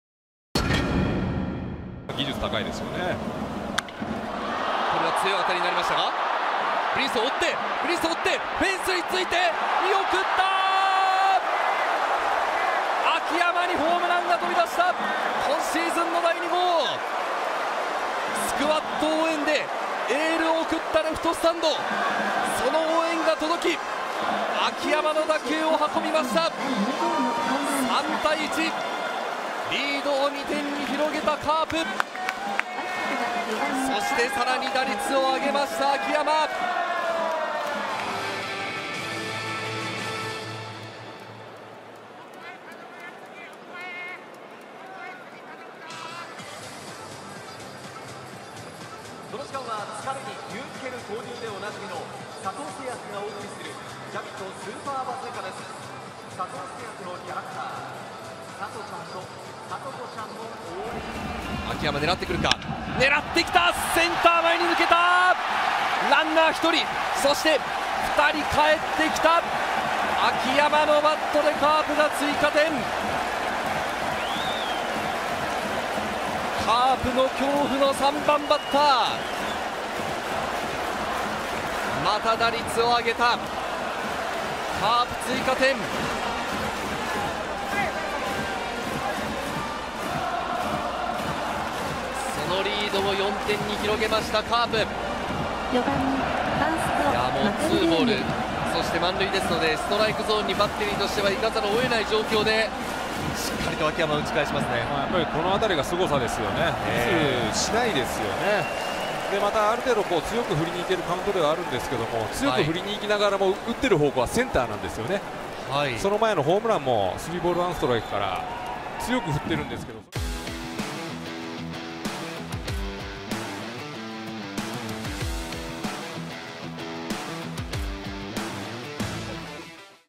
【秋山とまらん】秋山翔吾 本塁打を含むマルチ安打で未だに打率は4割!!
2023年4月30日(日)
「巨人×広島」 @東京ドーム